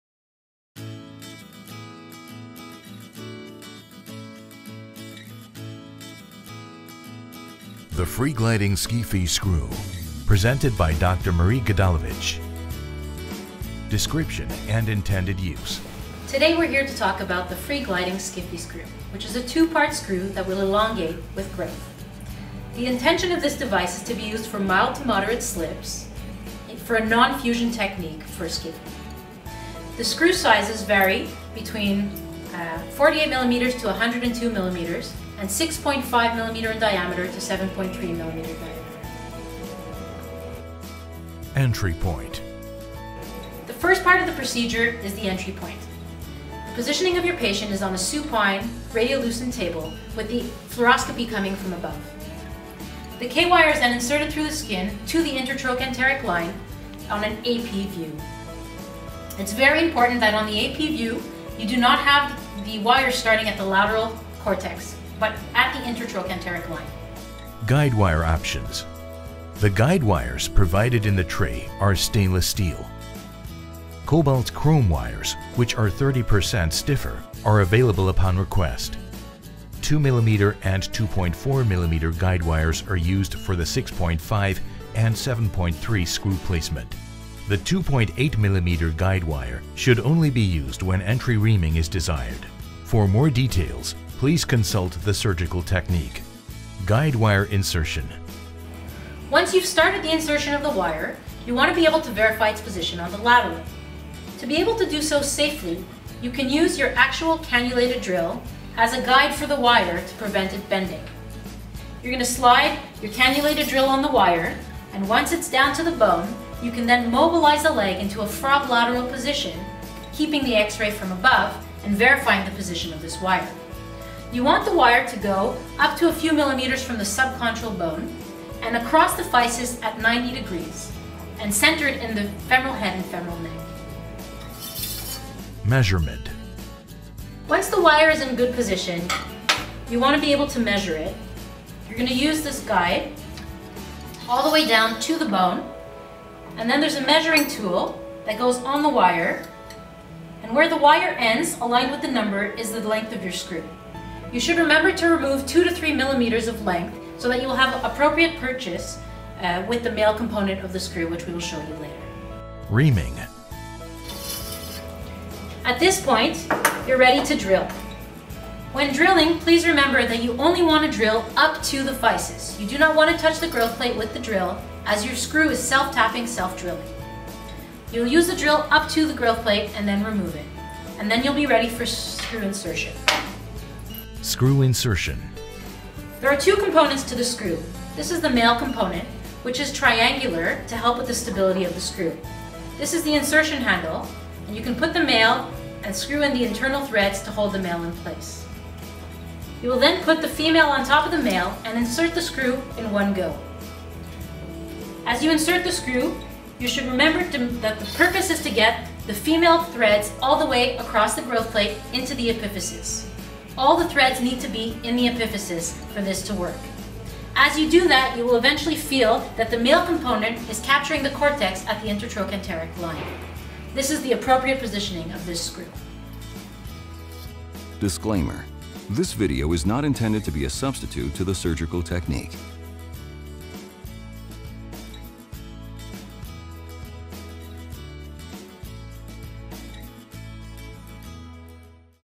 Free-Gliding SCFE Screw - Tips by Dr Marie Gdalevitch
Surgical technique walkthrough with Dr Marie Gdalevitch including tips and new guidewire options